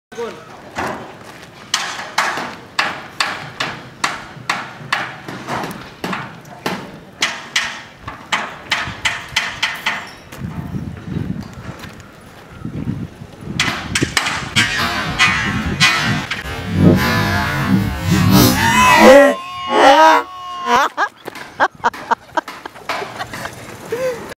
Photographer gets hit!
Just a small clip.
They probably didn't like what they saw in the camera!